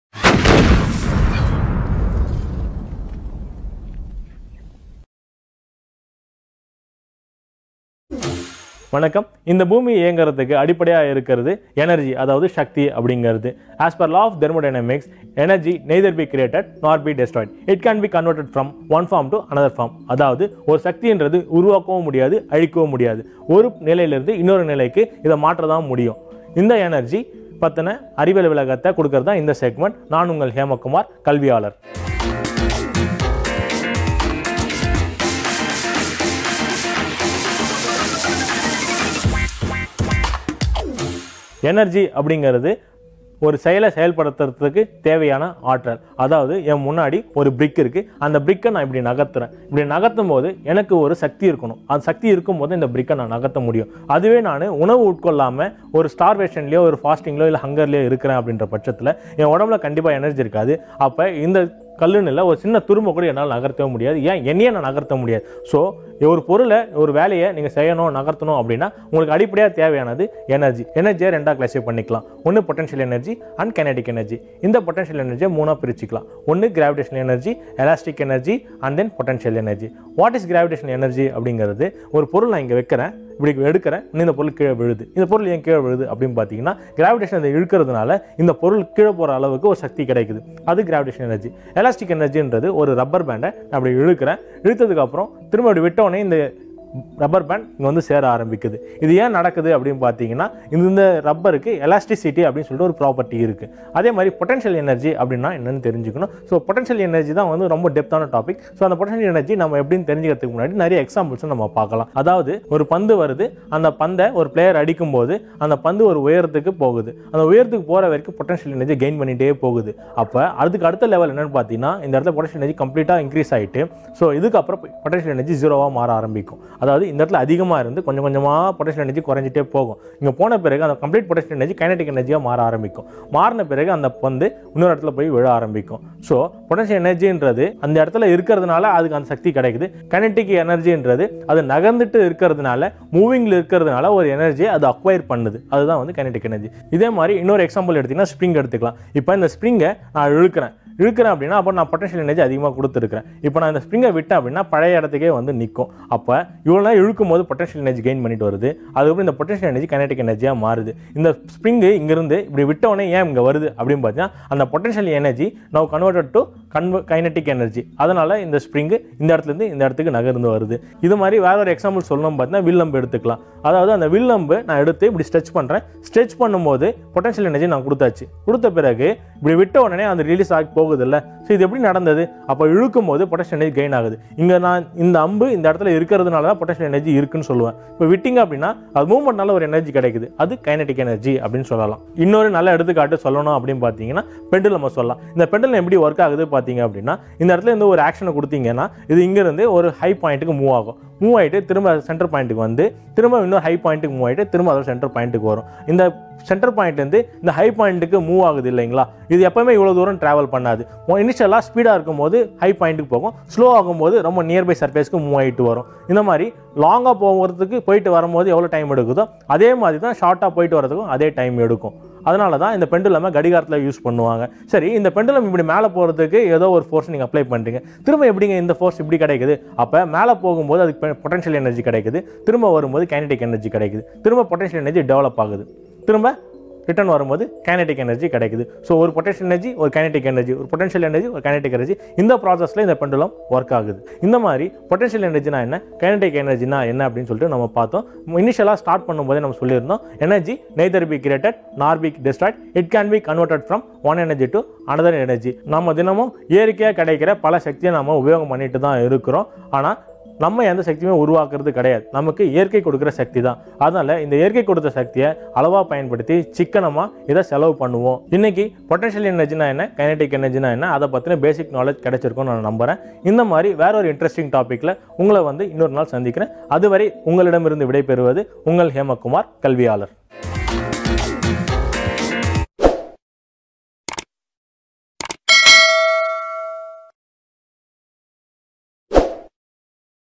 ஆற்றல் சக்தி மற்றும் இயக்க சக்தி - Potential energy and kinetic energy by Hemakumar.H
Potential energy is energy stored in an object due to its position or arrangement. Kinetic energy is energy of an object due to its movement - its motion.